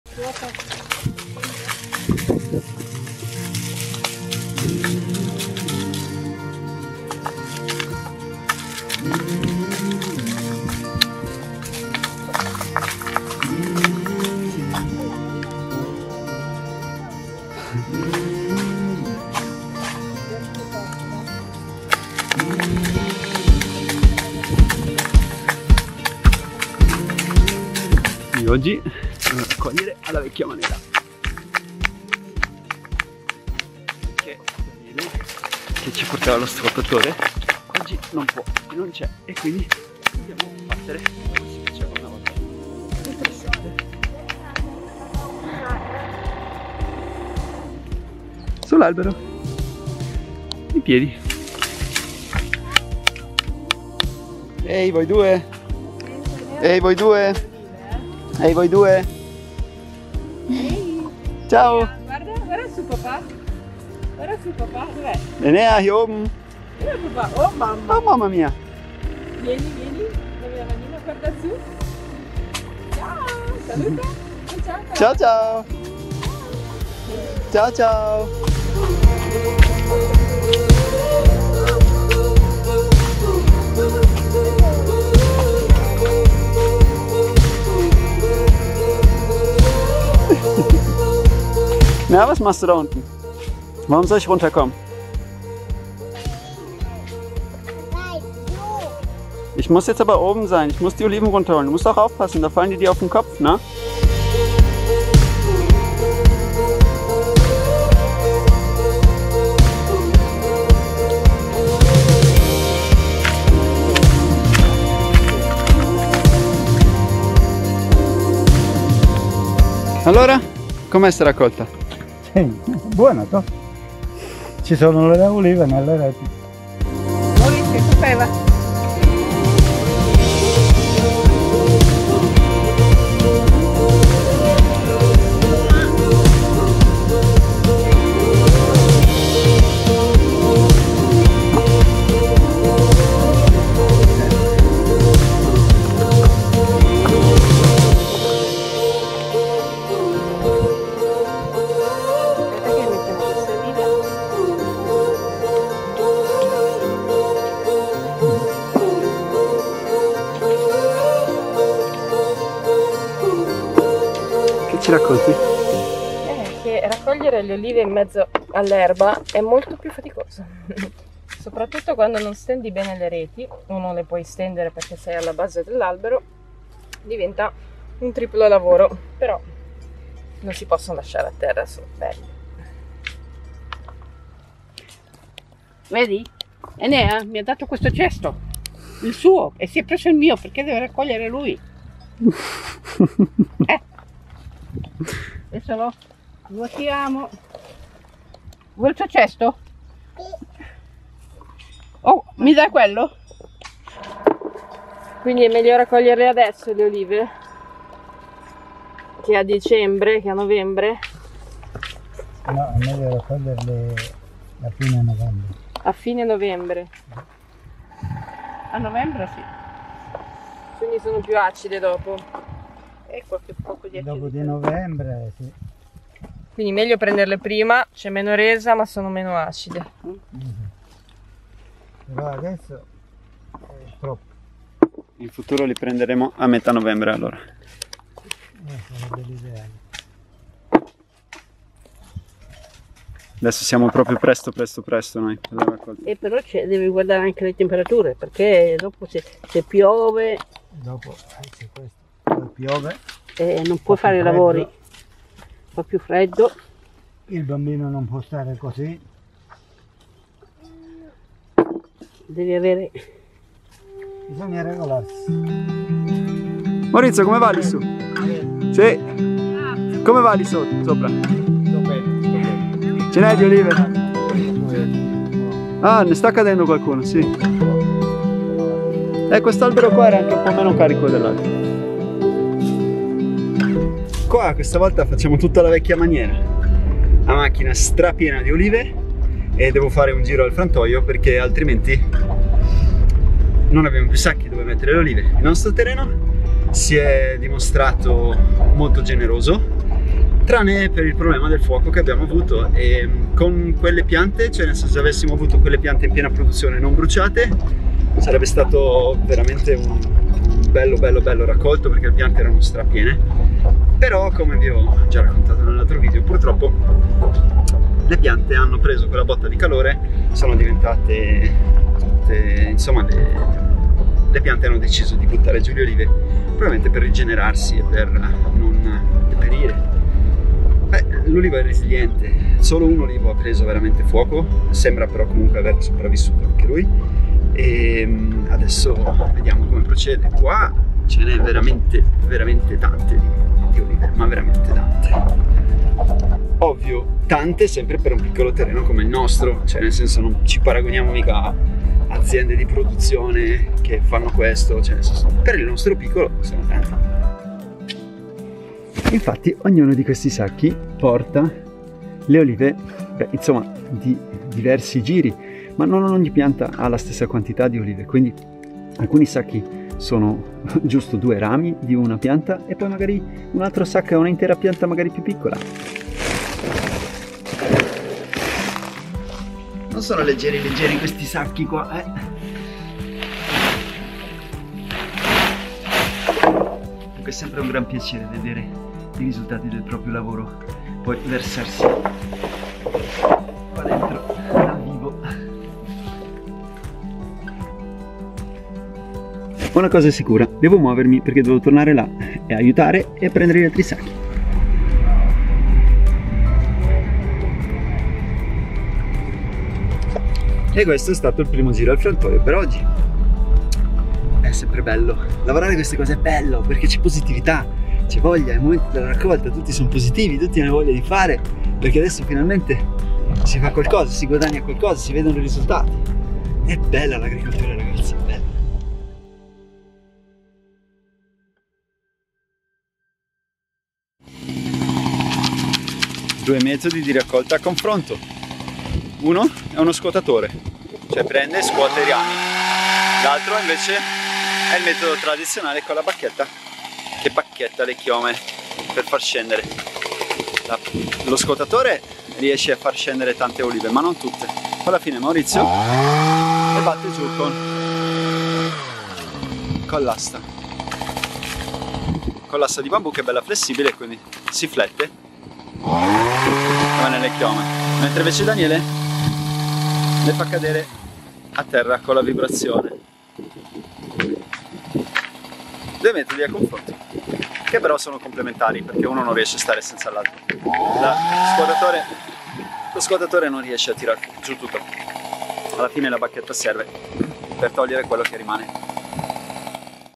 Raccogliamo le nostre olive | La Casetta
Il piccolo pezzo di terra intorno alla casetta è prevalentemente un uliveto. E' arrivato un momento tanto desiderato: raccogliamo per la prima volta le olive coltivate sul nostro terreno. Le nostre olive, per fare olio d'oliva extra vergine. Abbiamo temuto che il fuoco avesse distrutto tutto, ma in realtà, nel male le cose sono andate abbastanza bene. Con anche le piante compromesse il raccolto sarebbe stato eccezionale.

Non dimenticarti di accendere anche il campanellino per ricevere una notifica dei nuovi video. Mi trovi anche su